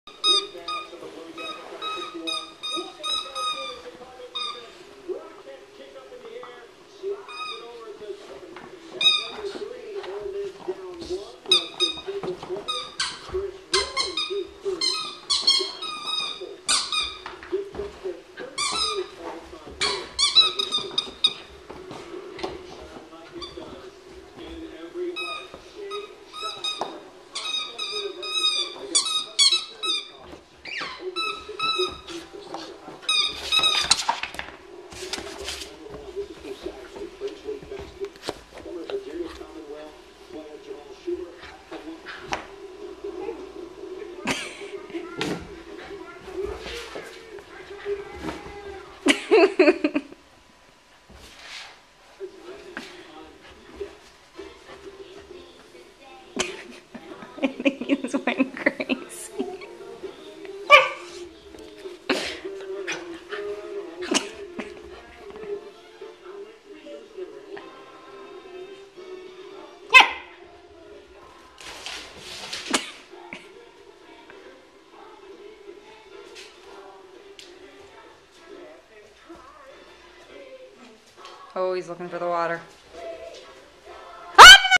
Baby Bear the Bernese Mountain Dog Bouncing About
Bernese Mountain Dog's first days in his new house with a lovely surprise ending.